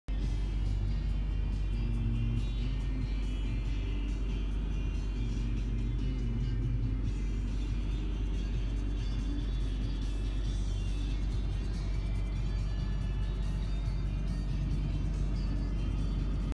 GovDeals: 2008 Chevrolet Silverado 3500HD LT1 4WD
2008 Chevrolet Silverado 3500HD LT1 4WD sold by Denver City and County, CO on GovDeals.com